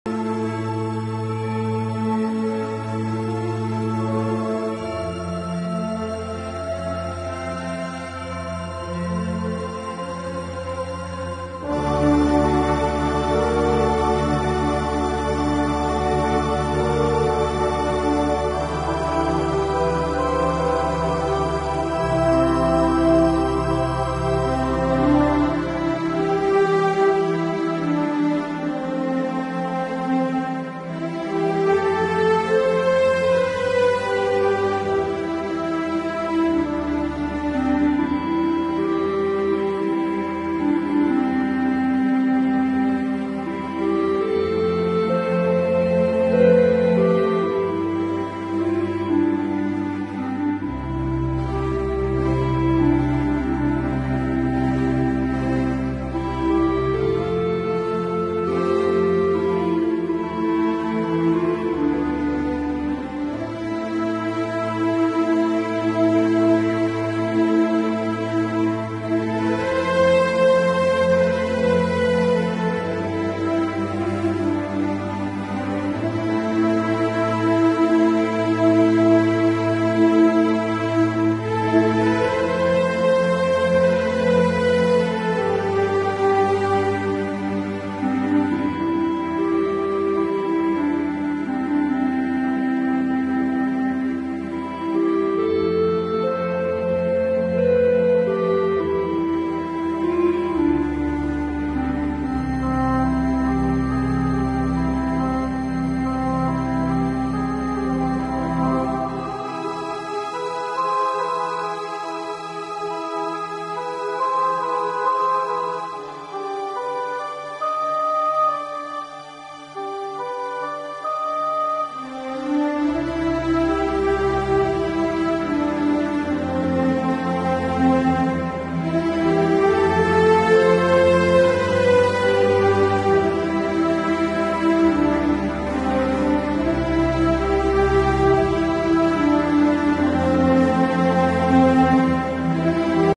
Red Deer Rutting in the Veluwe National Park
Impressive Red Deer Stag Rutting in  the Veluwe National Park, Holland